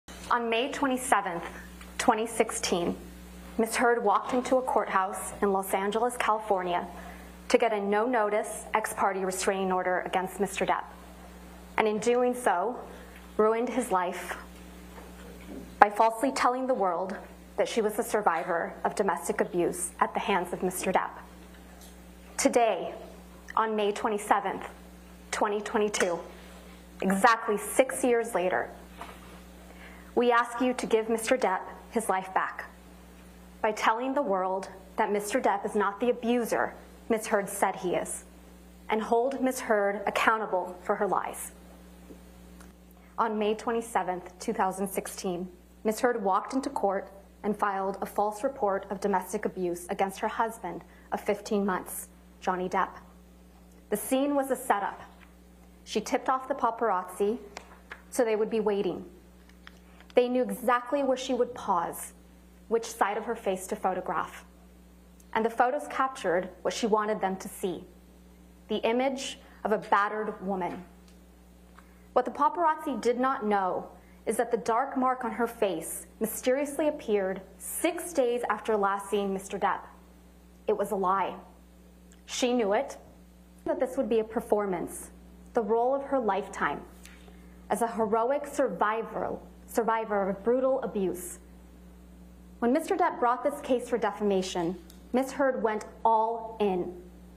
CAMILLE VASQUEZ acorrala a AMBER HEARD: "MINTIÓ, ESTA FUE LA ACTUACIÓN DE SU VIDA"
Camille Vásquez a Amber Heard: "ELLA MINTIÓ. FUE UNA ACTUACIÓN, LA ACTUACIÓN DE SU VIDA". Continúa el juicio por difamación que enfrenta a johnnydepp y AmberHeard. Este fue el cierre de argumentos de Camille Vásquez, abogada del actor en la recta final del juicio.